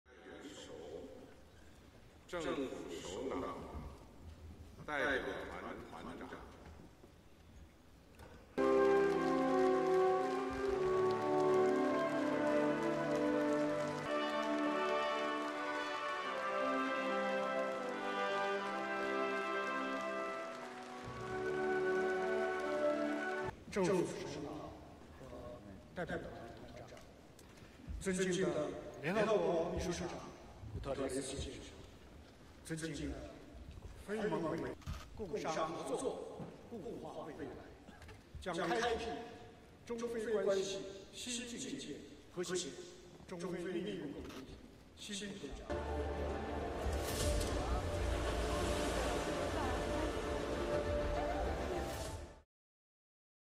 China's Xi presides over China–Africa Cooperation Forum opening ceremony | AFP
Chinese President Xi Jinping presides over the opening ceremony of the triennial Forum on China–Africa Cooperation. The summit is Beijing's biggest in years, with promises of cooperation in infrastructure, energy and education.